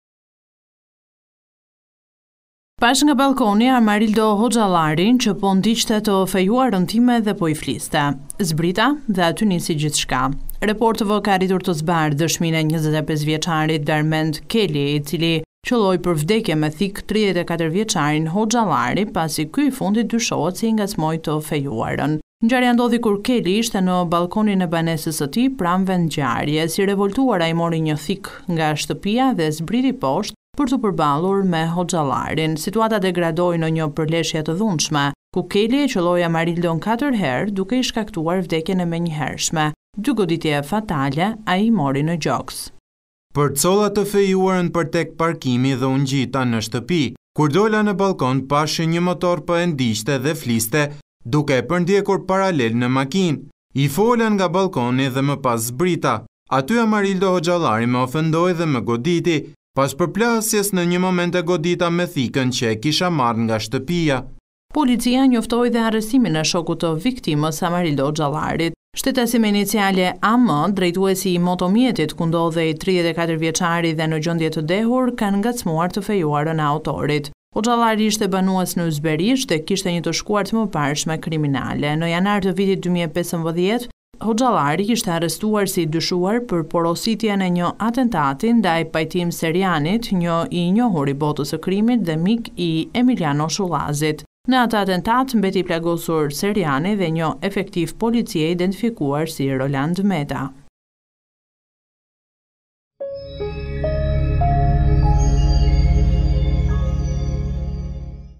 Vrasja në “Astir”, zbardhet dëshmia e Darmend Kelit: Amarildo më ngacmoi të fejuarën
Pas vdekjes së Amarildo Hoxhallarit nga 4 goditje thike në trup, oficerët marrin dëshminë e parë të 25-vjeçarit. Thotë se përcolli të dashurën dhe pa nga ballkoni se 34-vjeçari po e ndiqte me motor dhe po i fliste: “I fola dhe zbrita me thikën që mora në shtëpi, veprova në gjaknxehtësi!". Policia thotë se ndaloi dhe shokun 33-vjeçar të viktimës, drejtonte motorin i dehur